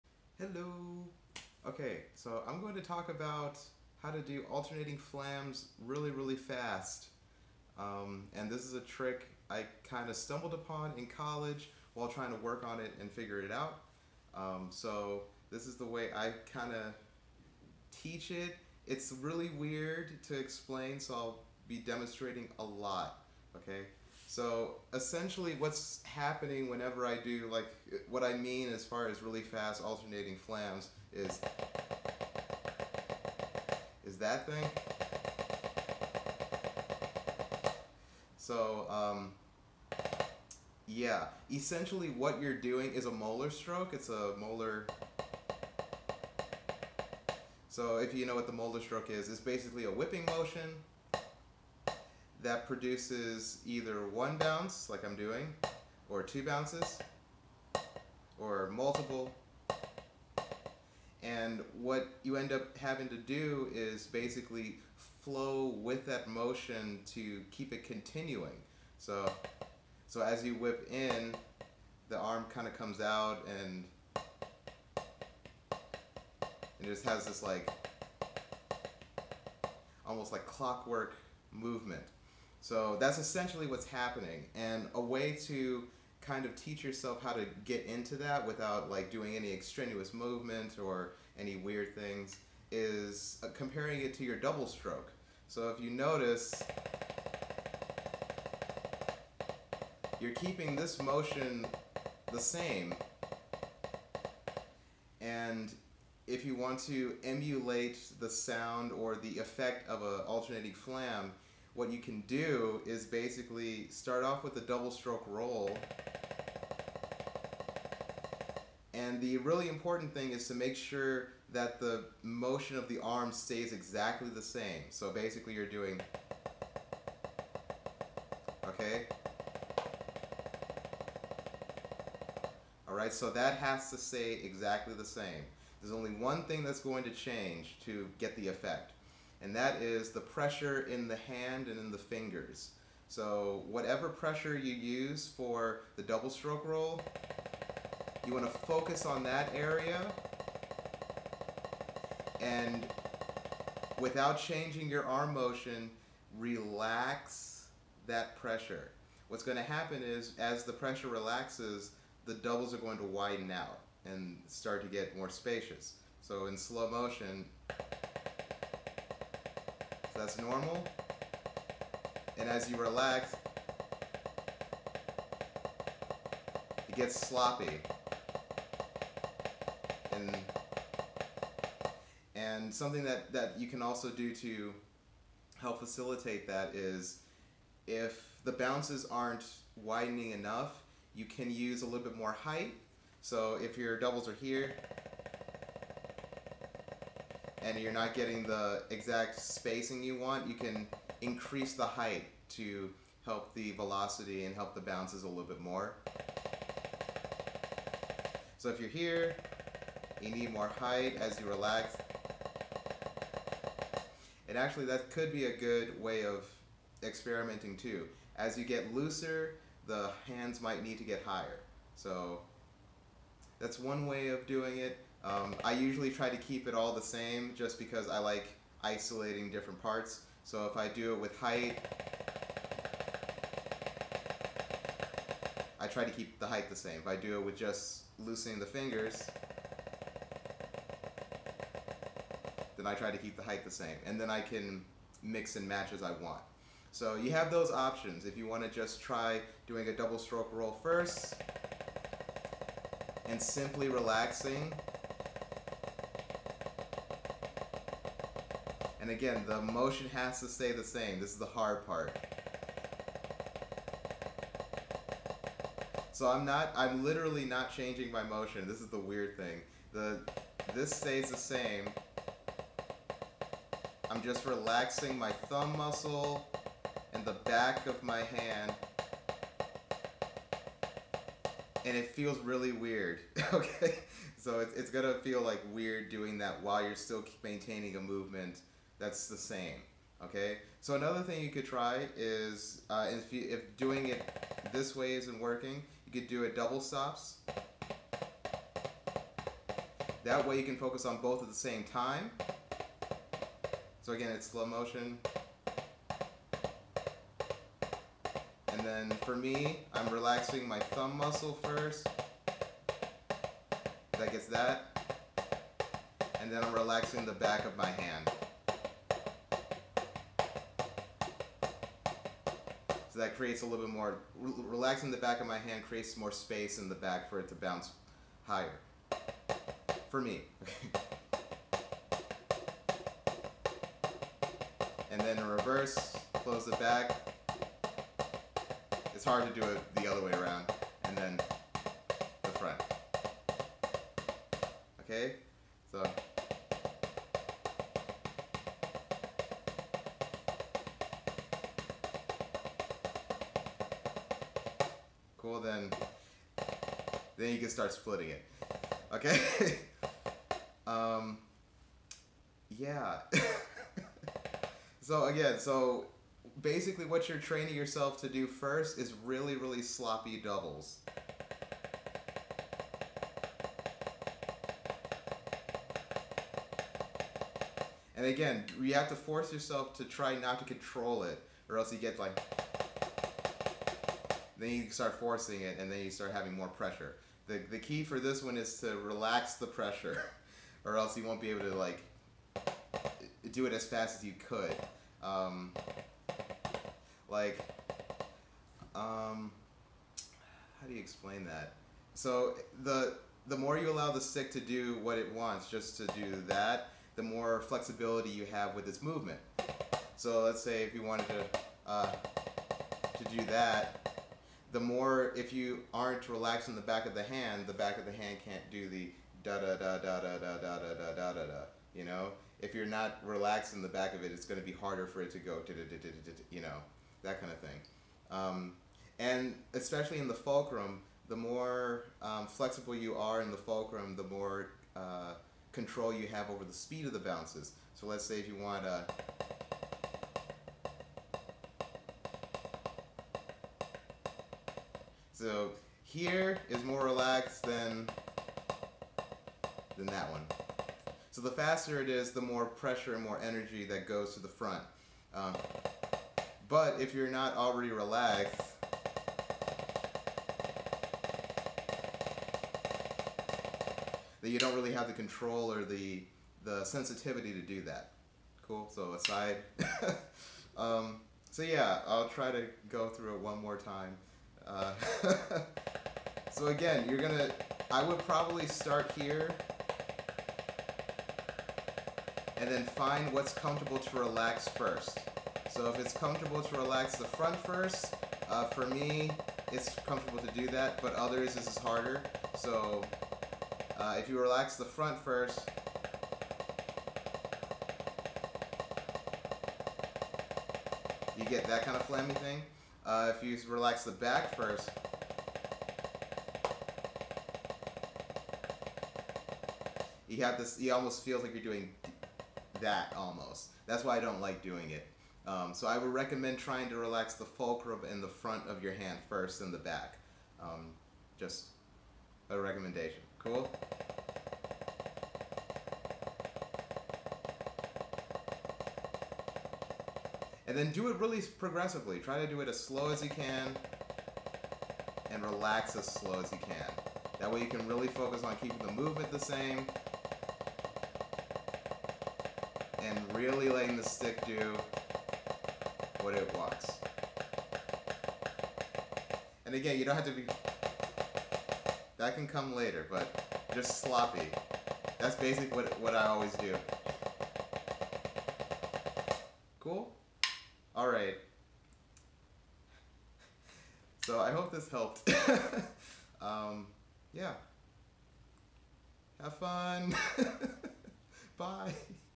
Alternating Flams Really Fast
Here's the way I stumbled upon a Moeller strokes alternating flams, making them a lot easier to do quickly.